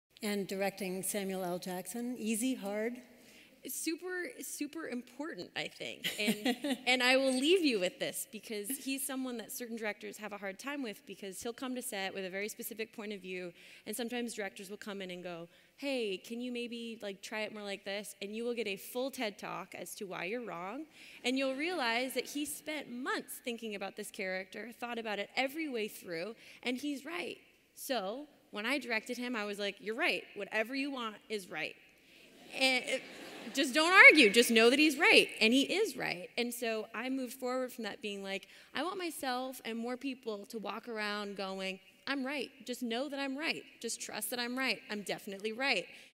Brie Larson: "I want myself and more people to walk around going: 'I'm right'"
Actor, director, and star of "Captain Marvel" Brie Larson shares a lesson in self-confidence she learned from actor Samuel L. Jackson while directing him on set.